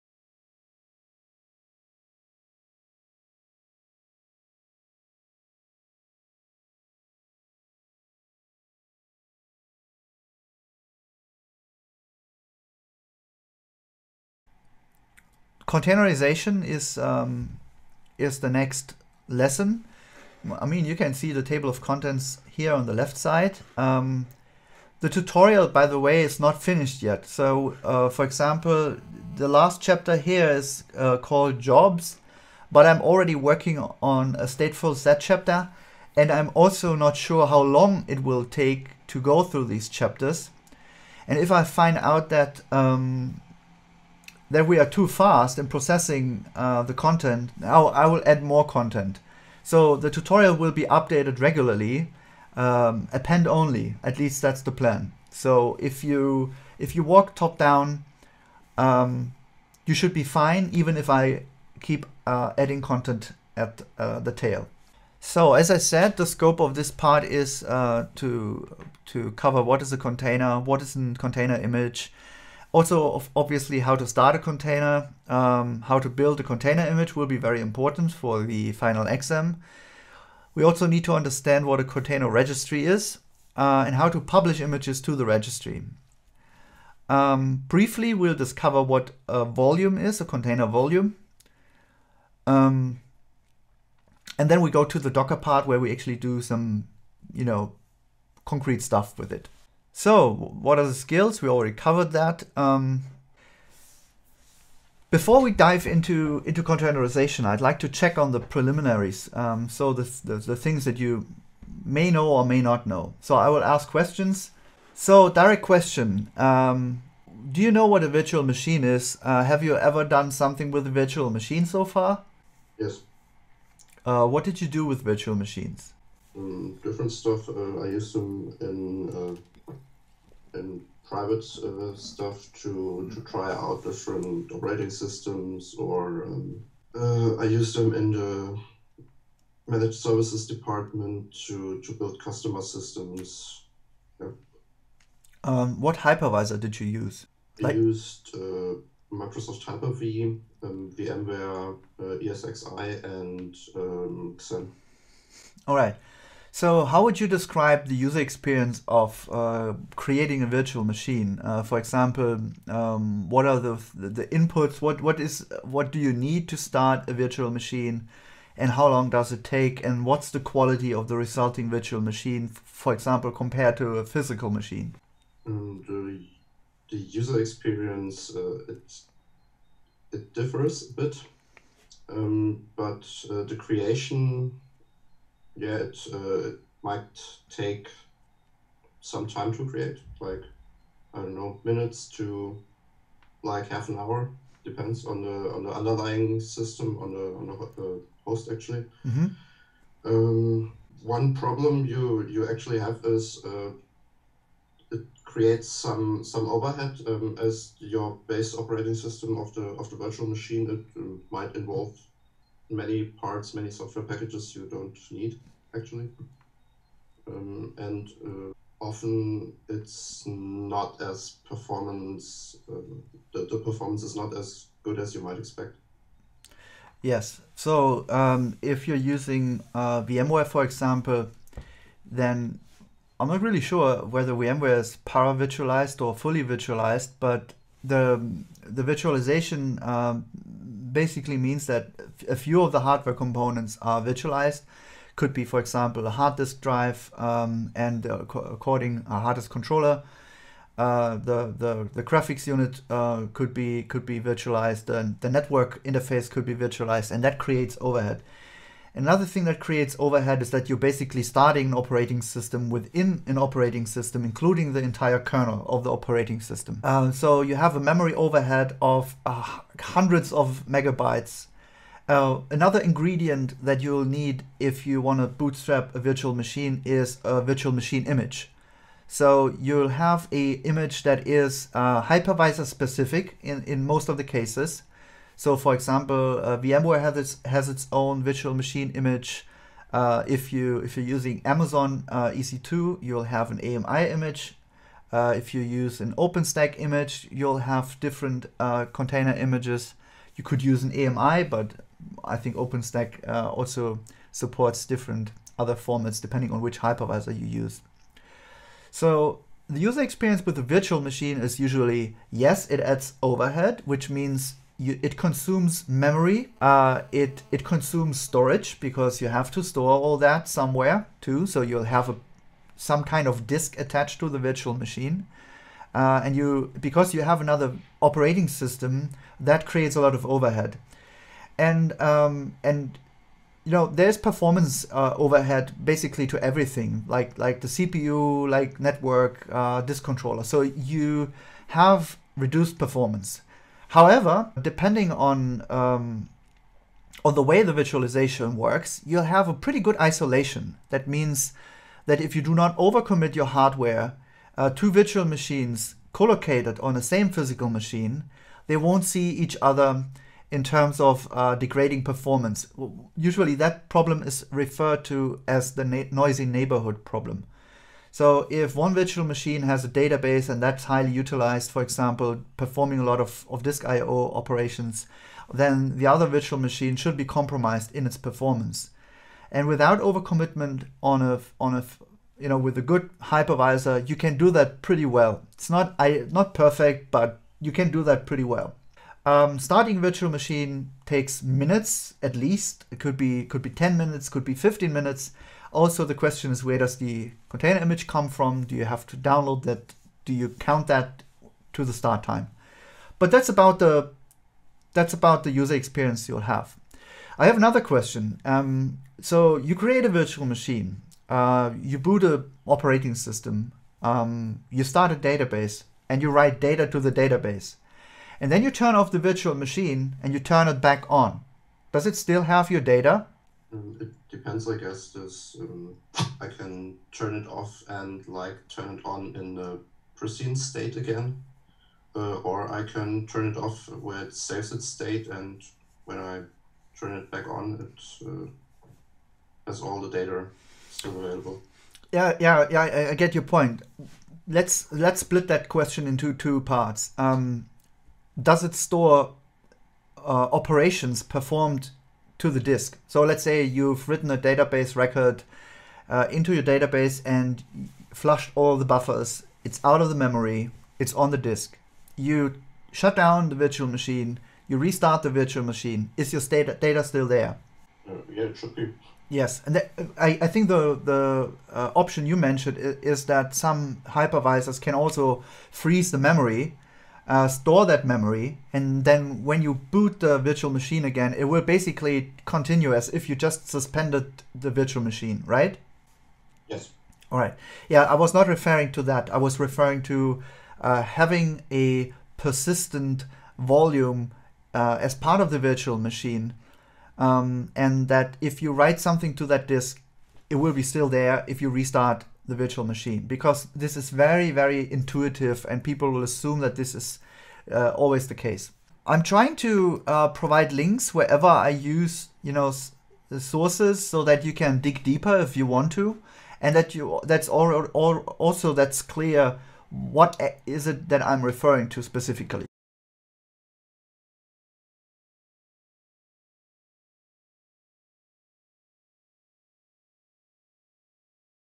Kubernetes Training #2 | Containerization · Container Overview
Learn in this video about the training structure. The goal of the Container lessons is to enable you as a developer to create container images as a preparation for the Kubernetes Training.

Therefore, the training will cover container basics and use Docker as the container image format. However, the training does not aim to enable developers to deploy application workloads to production environments using stand-alone Docker (in contrast to Docker as a container runtime in Kubernetes).